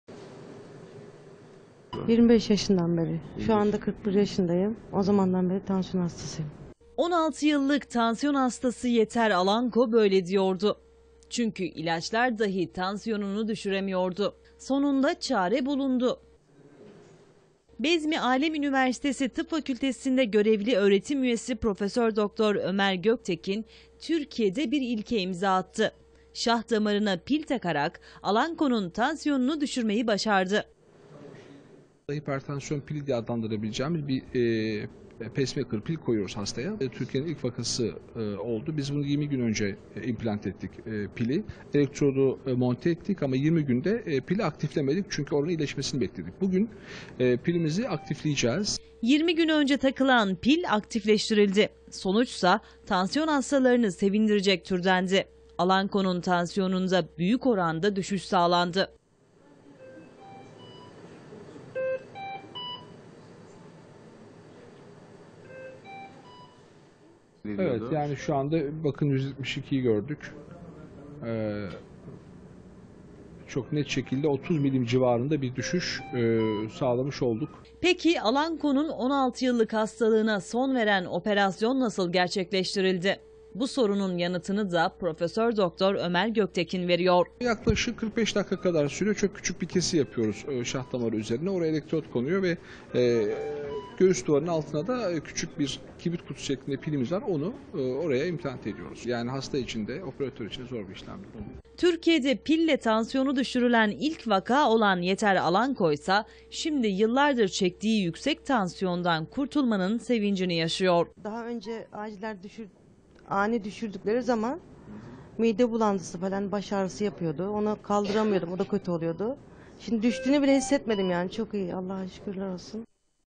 Bezmialem öğrencilerinden Türkiye'de bir ilk!
Bezmialem Üniversitesi Tıp Fakültesi Türkiye'de bir ilke imza attı. Tansiyonu ilaçlarla dahi düşmeyen Yeter Alanko'nun şah damarına pil takılarak tansiyonu düşürüldü. Peki bu operasyon nasıl gerçekleşti? İşte yanıtı